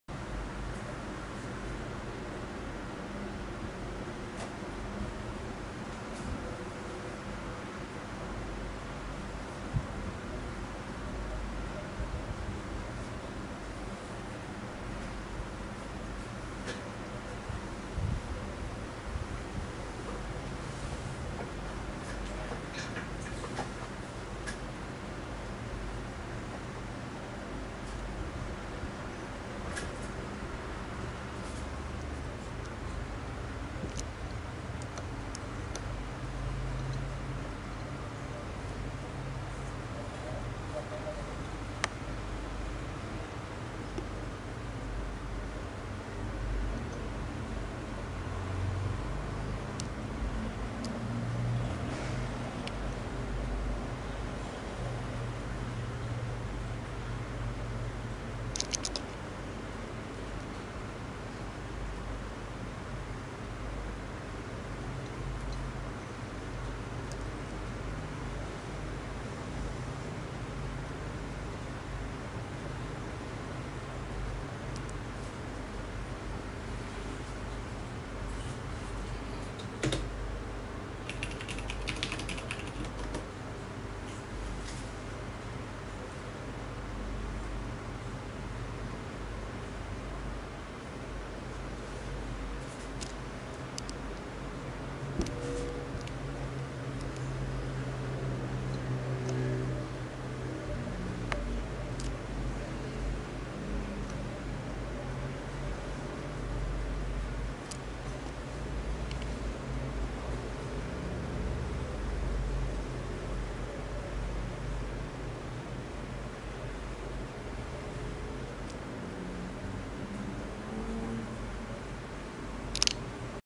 Nuc7i5bnh driving Predator X34 problem
This video is showing the green noise/artifacts when the Nuc is driving the Predator X34
Bios is 0049
Iris driver 4664

I don't see this problem when driving 4K TV